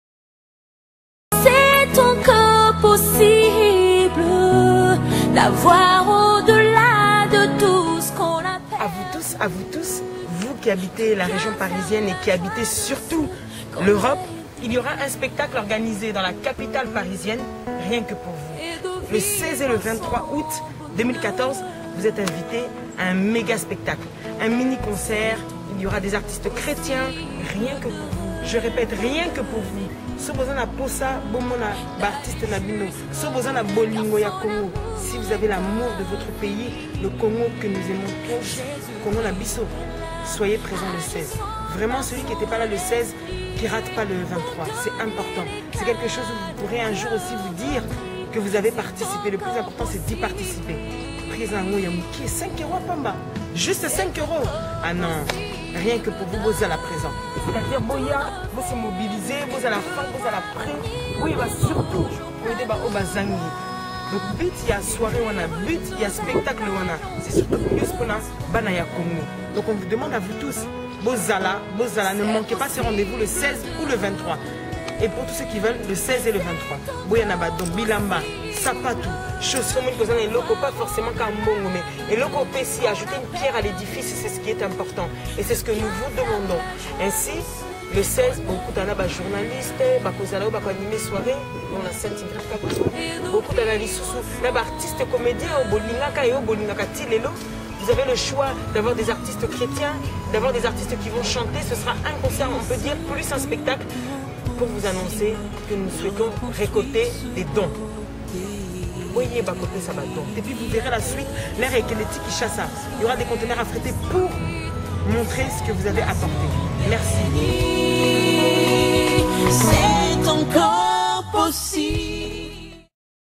Collecte de dons pour la RDC le 16 & 23 Août 2014 à Paris
Spectacle organisé pour les démunis au Congo.